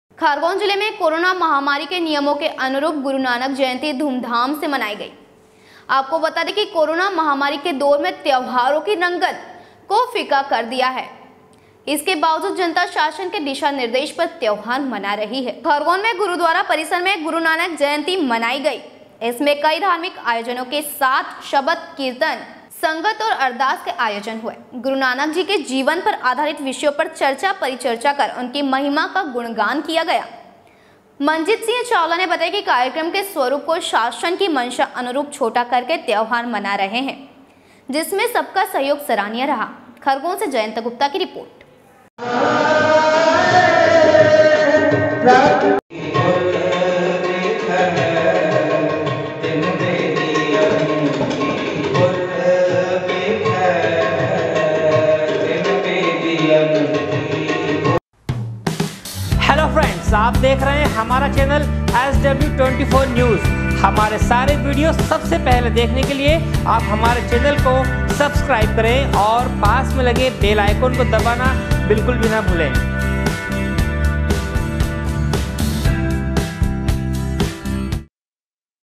खरगोन-जिले में कोरोना महामारी के नियमों के अनुरूप गुरुनानक जयंती धूमधाम से मनाई गई।
खरगोन जिले में कोरोना महामारी के नियमों के अनुरूप गुरुनानक जयंती धूमधाम से मनाई गई। आपको बता दे कि कोरोना महामारी के दौर ने त्योहारों की रंगत को फीका कर दिया है। इसके बावजूद जनता शासन के दिशा-निर्देश पर त्योहार मना रही है। खरगोन से जयंत गुप्ता की रिपोट